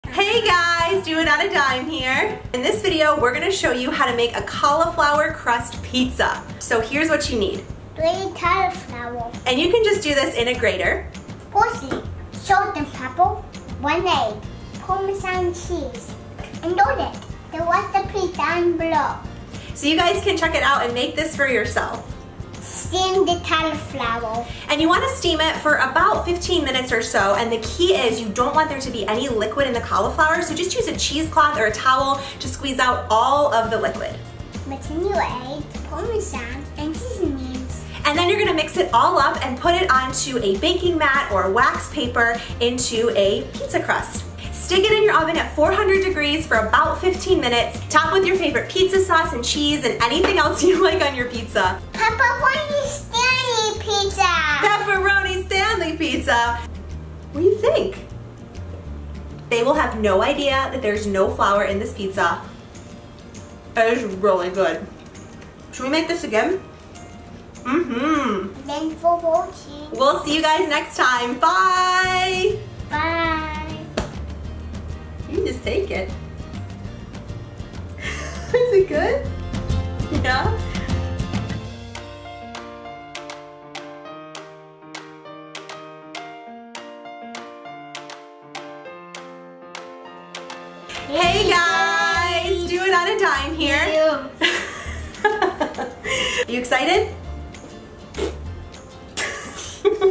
Cauliflower crust pizza! 🍕 Cooking with Carson
In this episode of cookingwithcarson we share how easy it is to make a gluten-free cauliflower pizza crust. This is the perfect lunch and snack for kids (and they won't have any idea they are eating veggies!) It's budget friendly and healthy. Hope you give it a try!

Follow the recipe above (make sure to really really squeeze out the water from the cauliflower!) and bake for 15 minutes at 400 degrees.  Add sauce, cheese, and desired toppings and bake again at 375 for about 5-7 minutes or until the cheese is melted.  Enjoy!

SilChef Silicon Baking Mat

P.S.  Yes, I am 40 weeks and 4 days pregnant in this video.  No baby, yet!

Write me at:
Kathryn "Doitonadime"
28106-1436